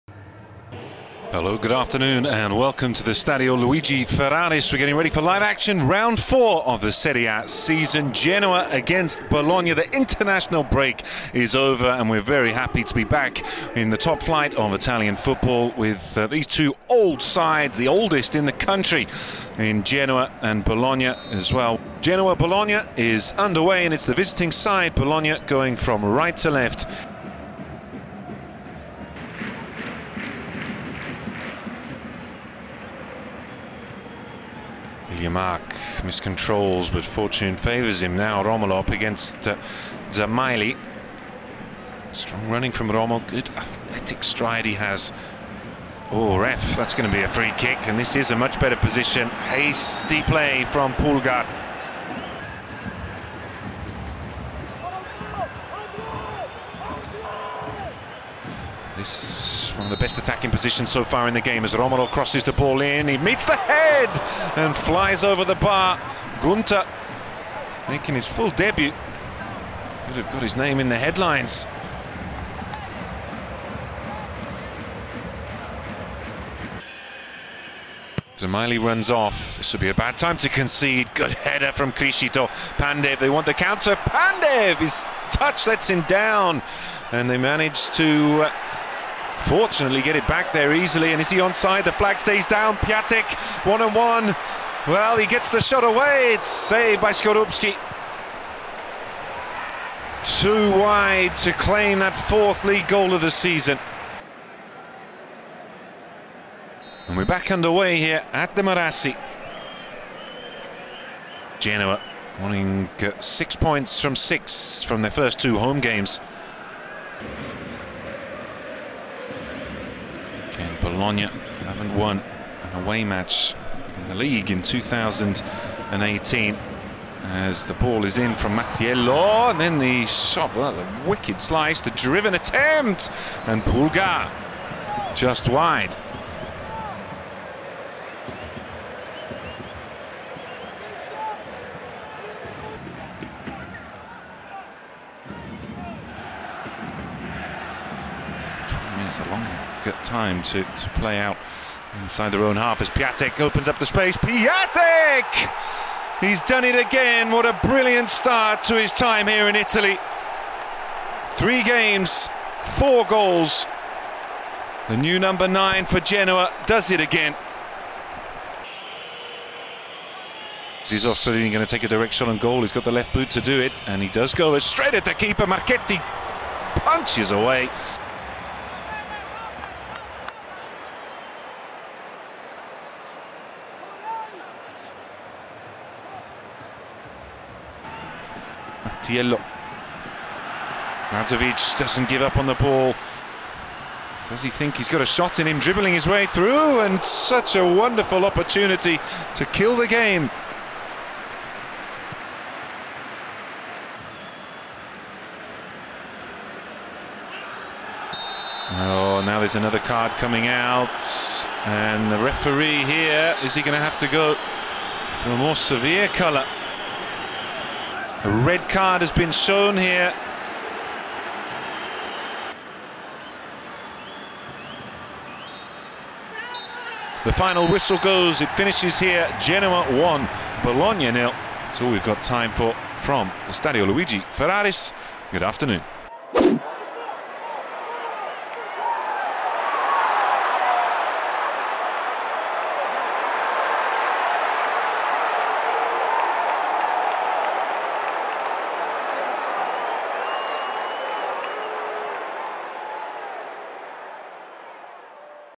Genoa 1-0 Bologna | Krzysztof Piątek Scores The Winning Goal For Genoa | Serie A
Krzysztof Piątek justt can't stop scoring!